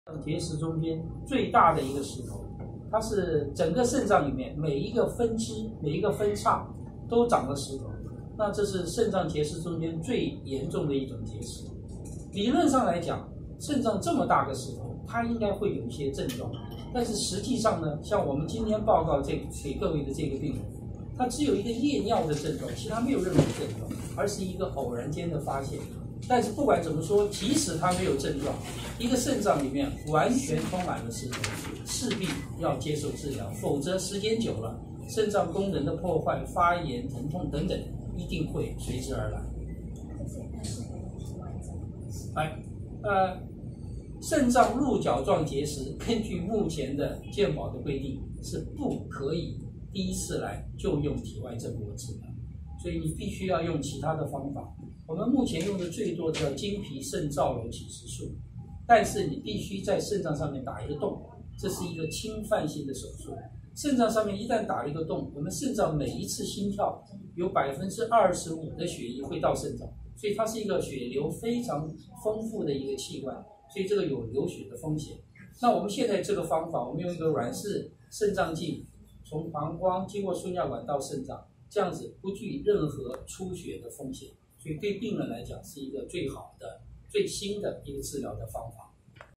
中年夜尿頻繁腎臟不聲不響長了5公分「鹿角結石」
「鹿角結石」45歲陳小姐，在中學擔任老師，平時除了有每天2次以上夜尿的問題，並沒有其他身體健康問題。但是，在一次健康檢查中，醫師發現陳小姐的腎臟有發炎的問題，照X光才發現她的左腎有滿滿的結石，由於沒有發燒、腰痛等症狀，她的結石發展成5公分以上，且問題已經存在2年以上。